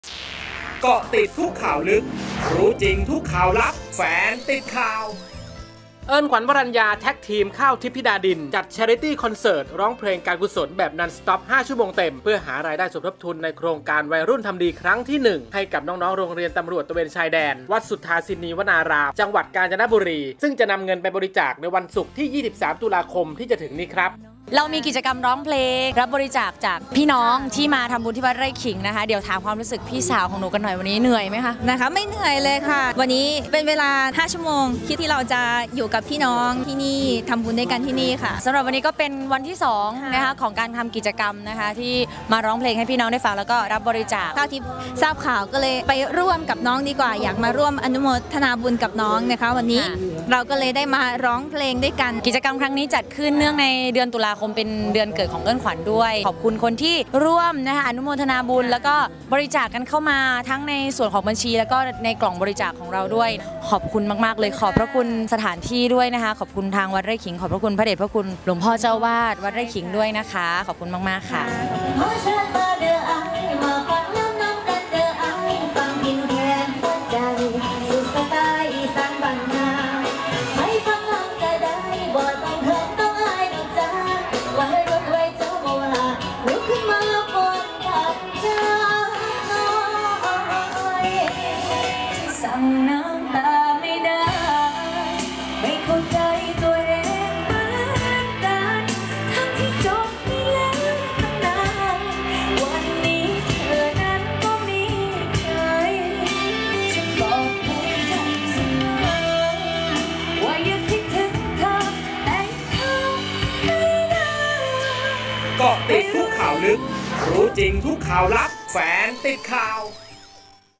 【ข่าว】"เอิ้นขวัญ วรัญญา , ข้าวทิพย์ ธิดาดิน" ร้องเพลงหาเงินมอบโรงเรียนตำรวจตระเวนชายแดน จ.กาญจนบุรี
"เอิ้นขวัญ วรัญญา , ข้าวทิพย์ ธิดาดิน" จัดคอนเสิร์ตการกุศล ร้องเพลงหาเงินมอบโรงเรียนตำรวจตระเวนชายแดน จ.กาญจนบุรี ในโครงการวัยรุ่น..ทำดี ครั้งที่ 1 ณ วัดไร่ขิง จ.นครปฐม